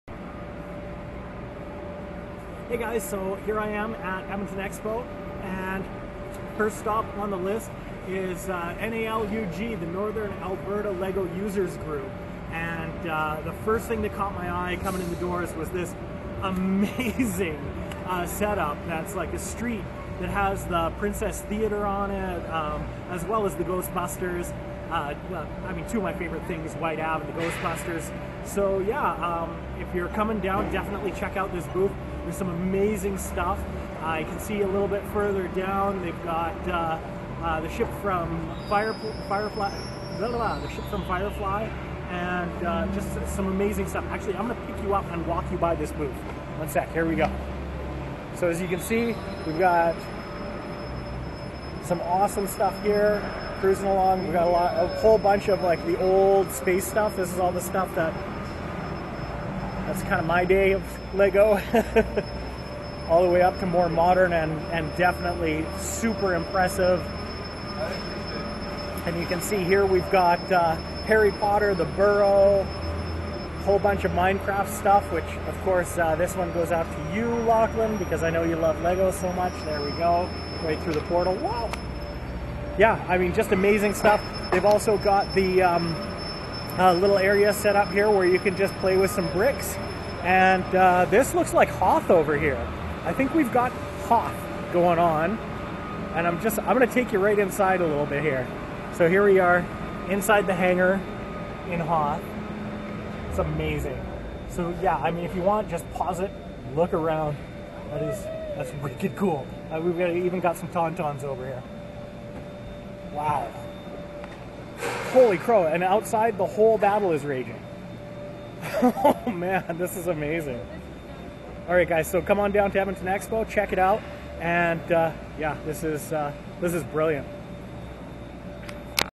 NALUG - Northern Alberta Lego User Group at EdmontonExpo 2016
Amazing builds here, I LOVE the Empire scene they built.